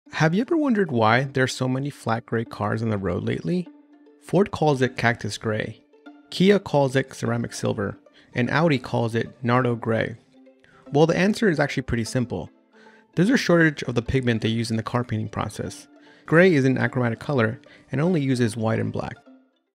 Why Is Every Car on the Road Grey?
Every other car you see today seems some shade of gray. How much truth is there to this, and how far back does the trend of achromatization of our cars actually go? Let’s take a look.